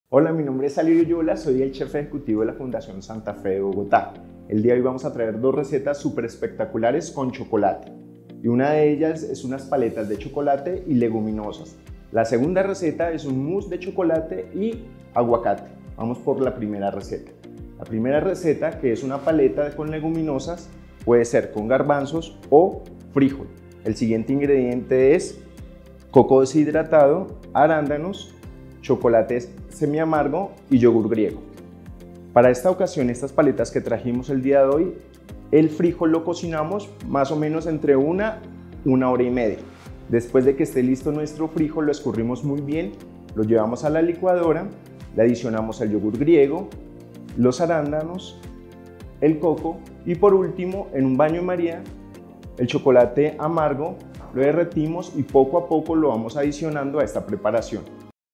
Receta Paleta con Leguminosa y Mousse de Chocolate y aguacate
Aprovechando los beneficios del chocolate, te compartimos 2 diferentes recetas con este delicioso alimento, que se relaciona con nuestras emociones, ya que estimula la producción de endorfinas y serotoninas que nos hacen sentir felices.